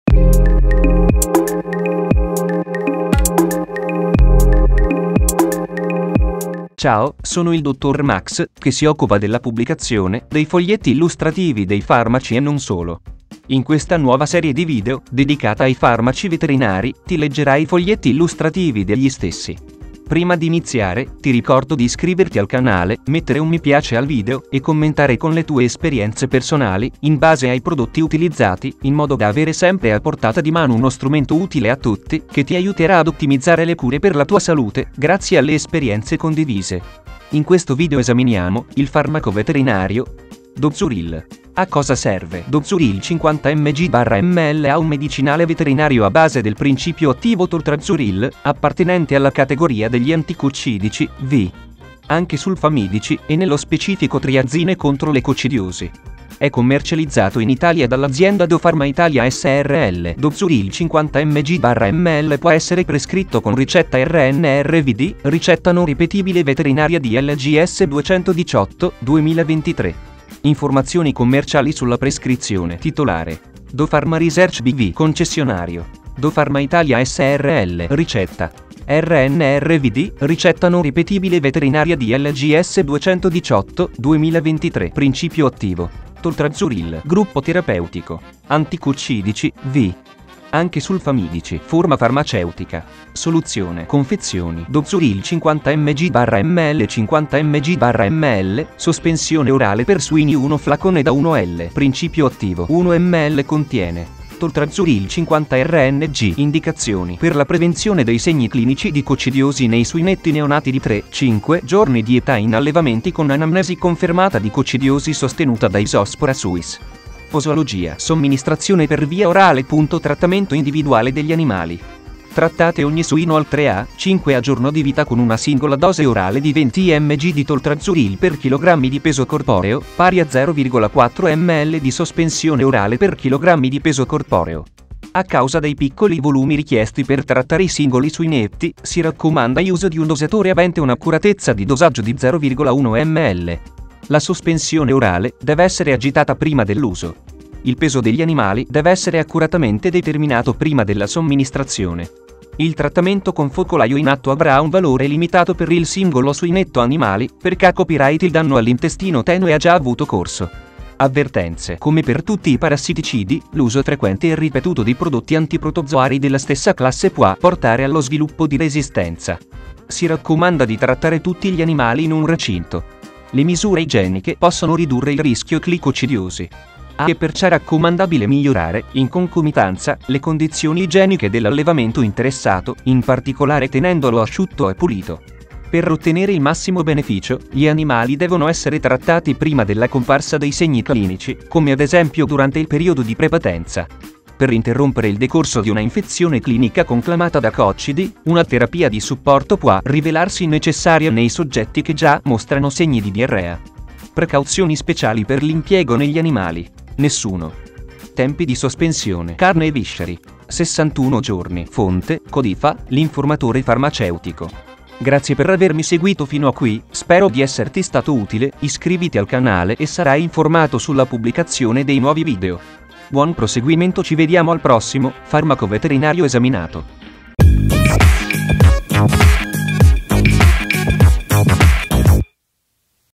🐶 DOZURIL Farmaco Veterinario a cosa serve ? - Foglietto Illustrativo Salute
Abbiamo letto il Foglietto Illustrativo e parlato del Farmaco Veterinario: DOZURIL

Capitoli ⏱
0:00 Introduzione
0:06 Presentazione
0:40 Farmaco Veterinario DOZURIL
1:01 A cosa serve ?

DOZURIL Farmaco Veterinario